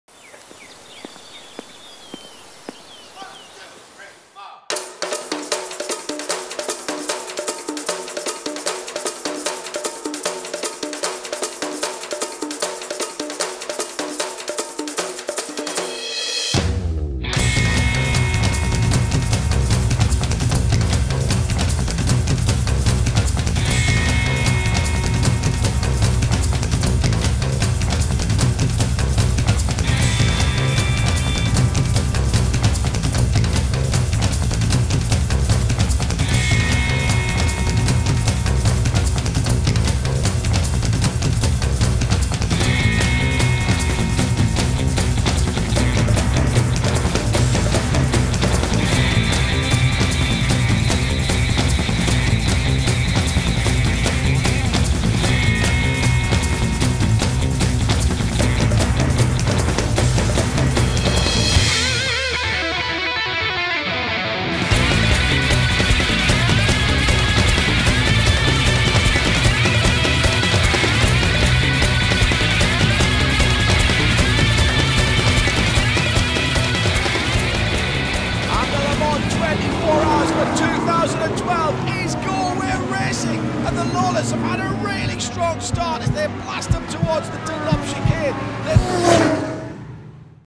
LOLA How To Build a Le Mans Car from scratch In 90 Seconds music.mp4
How to build a Le Mans car in 90 seconds. from scratch. Just add Lola Magic! - A production by Beagle Media for Lola Group.